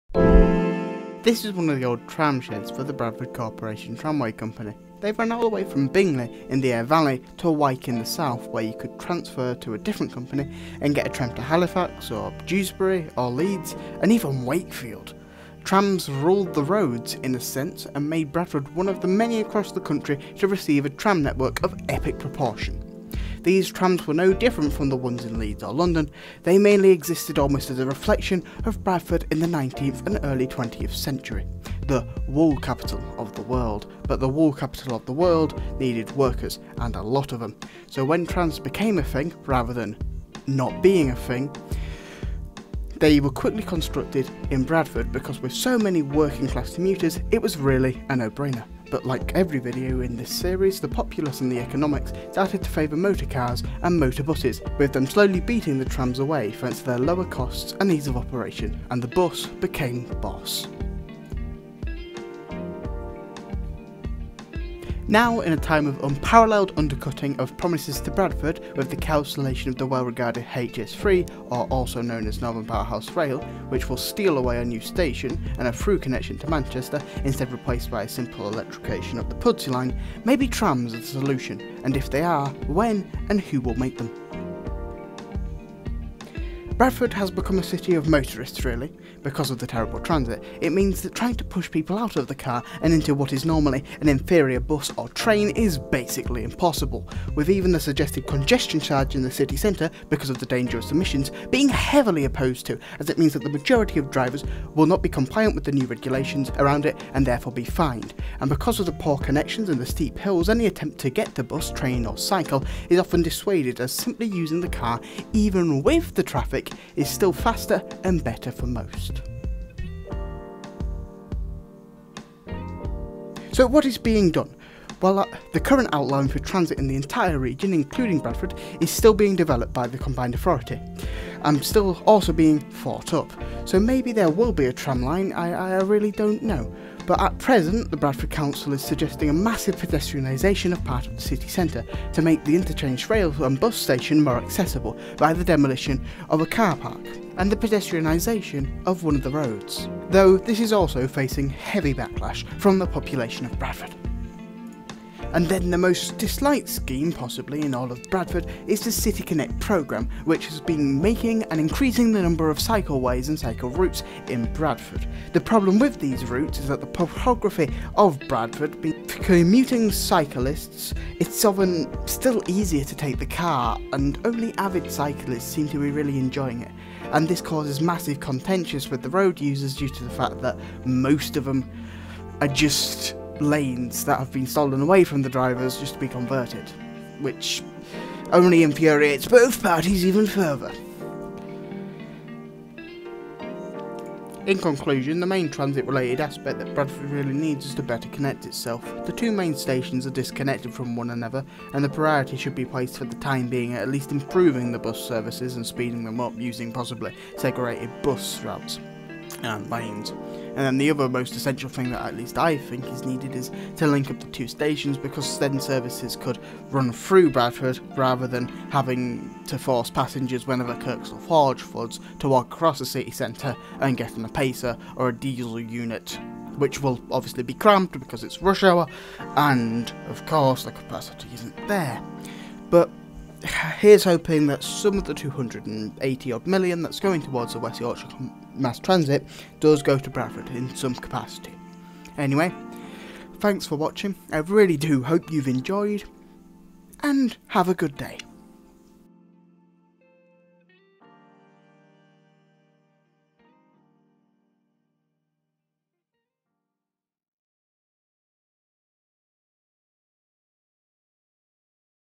Bradford's Transit Past and Future | Clarified
Bradford in the North of England near Leeds is one of the youngest cities in the Uk, though it faces a drought of investment into Transit. The Old City of Bradford used to maintain a tram network much like all the other major urban centres of the time to transport the working class to the mills and back! Though now after the continued betrayal of the Northern Powerhouse Rail plans does the future hold any good news for Bradford?

Trams similar to Trains; in the North of England, were replaced by buses and became the main form of transit using the infrastructure to provide transportation. Leeds and Bradford are close within West Yorkshire under the Combined Authority. Also, if any of you want to support me look here: